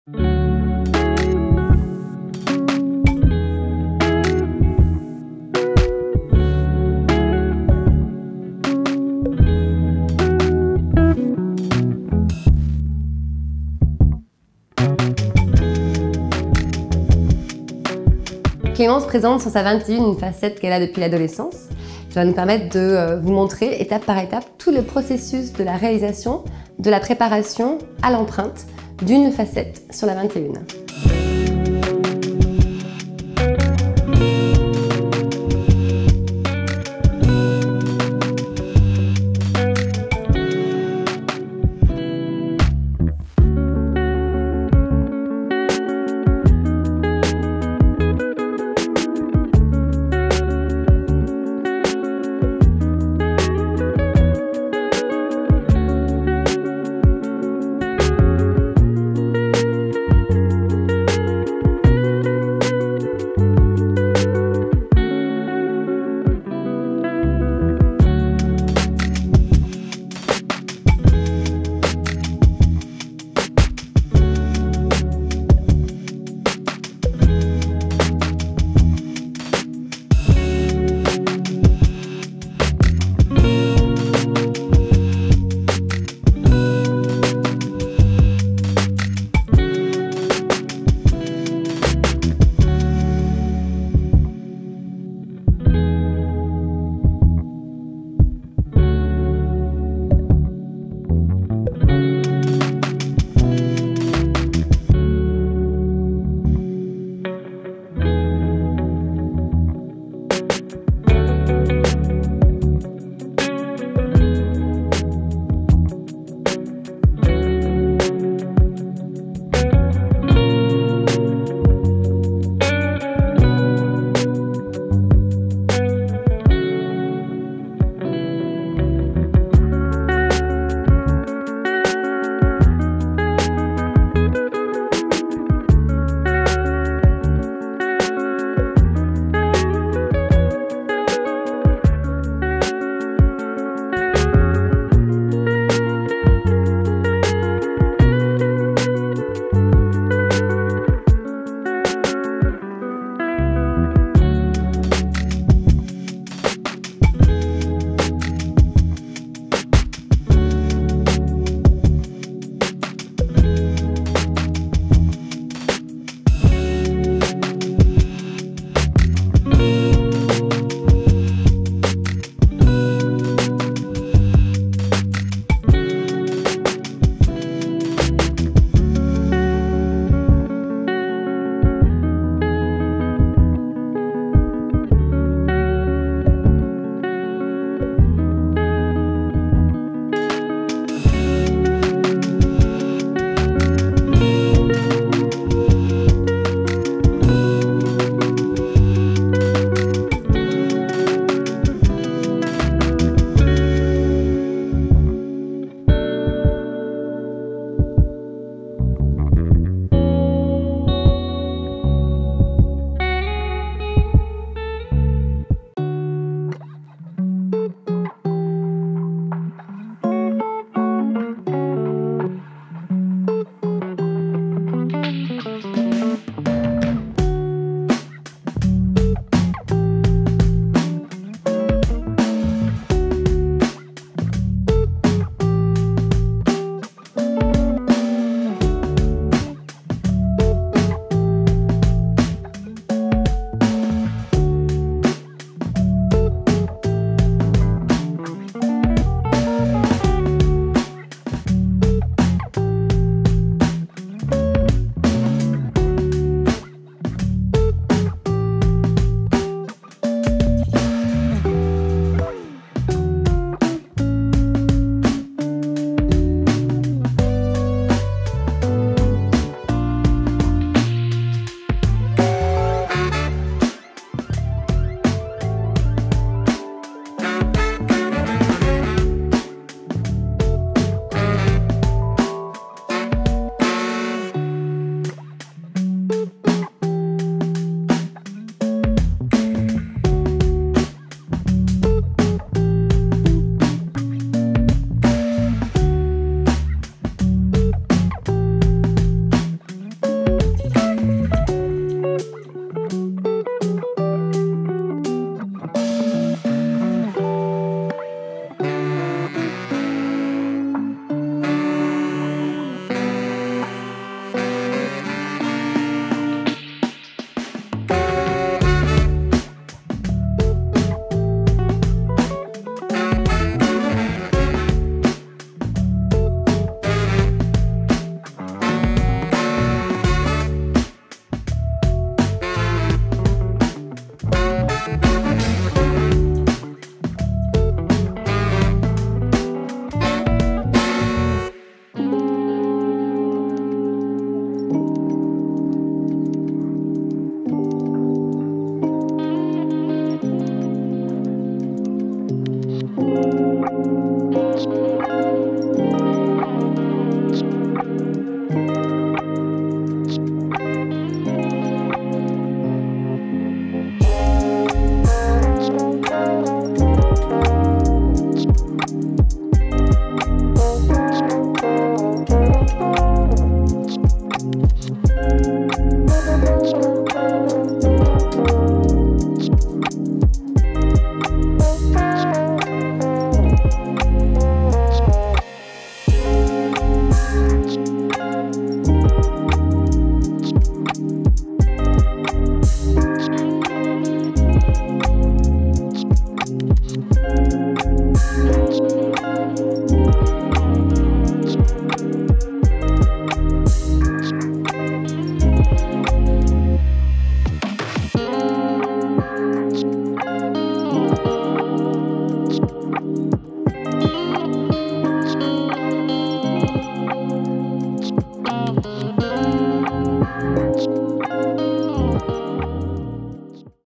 📹 Tuto clinique #02 - Réfection d'une facette unitaire
🦷 Dans cette nouvelle vidéo, le Dr Linda Martin réalise toutes les étapes de préparation et de prise d'empreinte pour la pose d'une facette en céramique.

📱 Suivez-nous !

📹 Clinical tutorial #02 - Refurbishing a single-unit veneer

🦷 In this new video, Dr. Linda Martin performs all the preparation and impression-taking steps for the placement of a ceramic veneer.